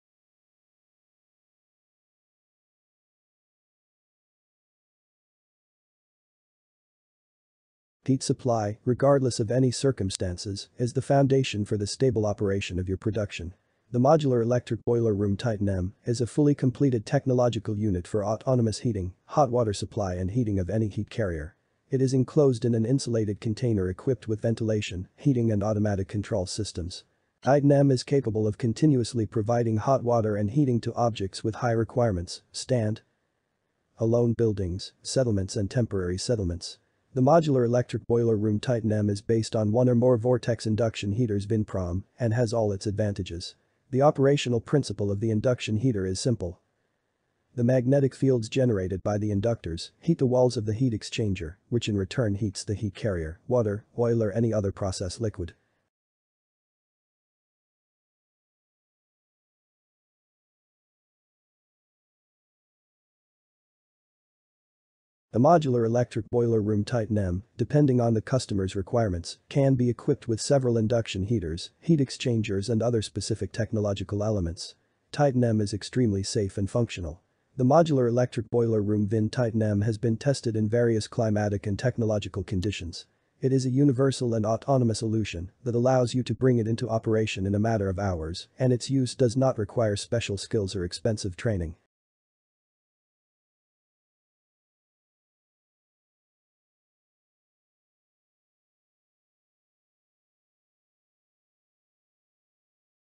MODULAR ELECTRIC BOILER ROOM «TITAN-M»
Heat supply, regardless of any circumstances, is the foundation for the stable operation of your production.

The modular electric boiler room Titan-M is a fully completed technological unit for autonomous heating, hot water supply and heating of any heat carrier. It is enclosed in an insulated container equipped with ventilation, heating and automatic control systems. Titan-M is capable of continuously providing hot water and heating to objects with high requirements — stand-alone buildings, settlements and temporary settlements.

The modular electric boiler room Titan-M is based on one or more vortex induction heaters VIN-Prom, and has all its advantages.

The operational principle of the induction heater is simple - the magnetic fields generated by the inductors heat the walls of the heat exchanger, which in return heats the heat carrier: water, oil or any other process liquid.

The modular electric boiler room Titan-M, depending on the customer's requirements, can be equipped with several induction heaters, heat exchangers and other specific technological elements. Titan-M is extremely safe and functional.

The modular electric boiler room VIN Titan-M has been tested in various climatic and technological conditions. It is a universal and autonomous solution that allows you to bring it into operation in a matter of hours (minutes), and its use does not require special skills or expensive training.